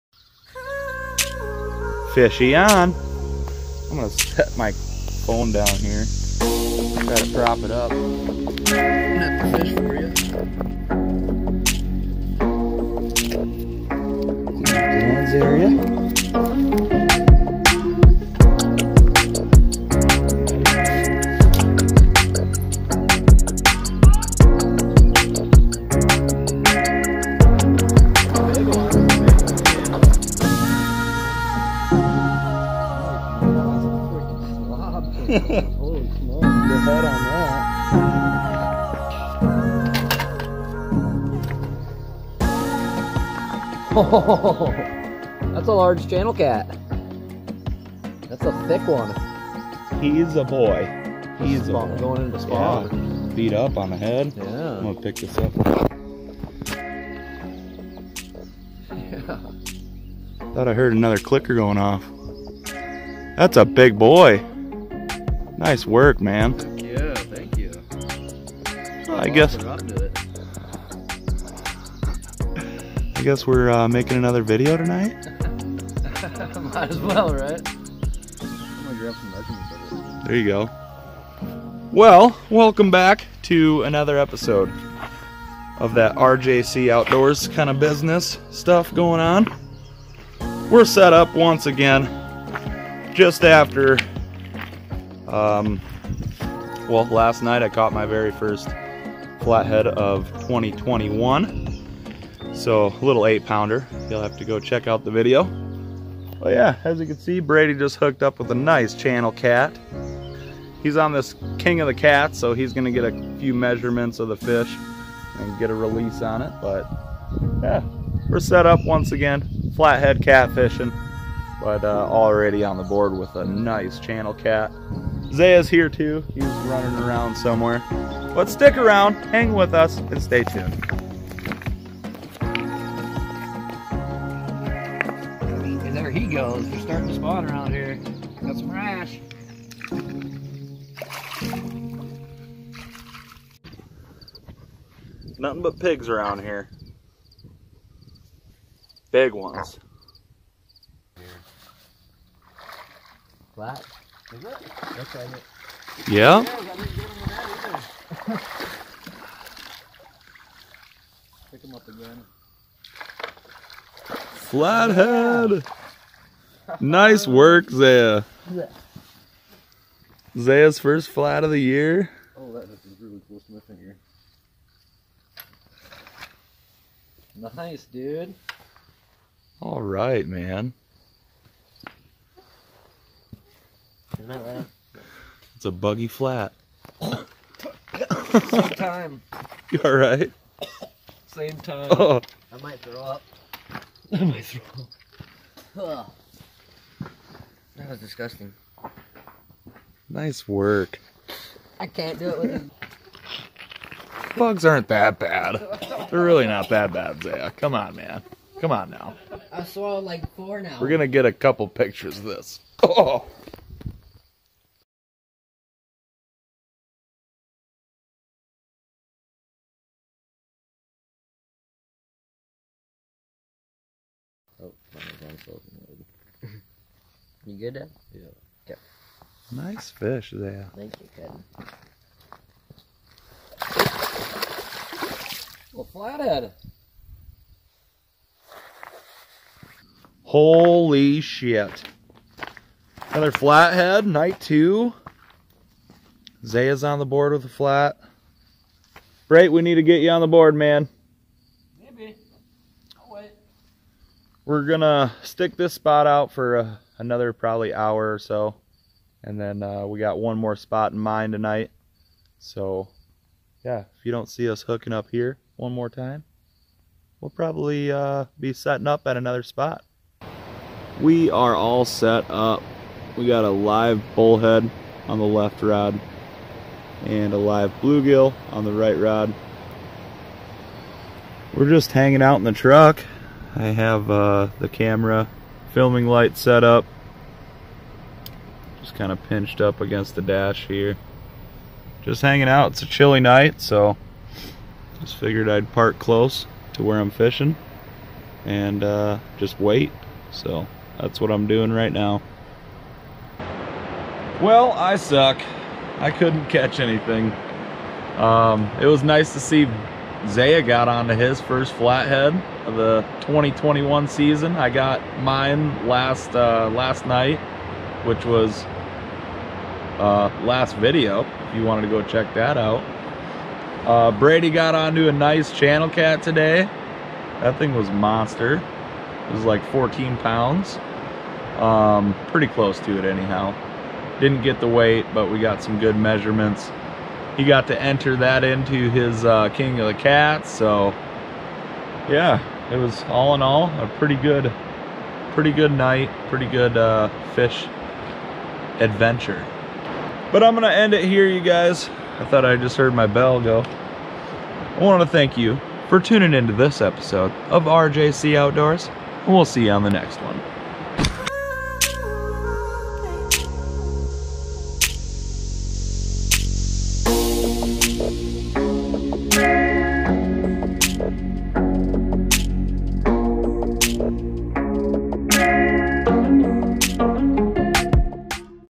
Catfish like buggy nights because bait fish Love buggy nights
Unexpected video here!! I didn't plan on filming but decided to pull the phone out of my pocket and start recording.  Brady started the day out with a nice channel cat and Isaiah ended the night with his 1st flat head of the 2021 season
(- (Social Media) -)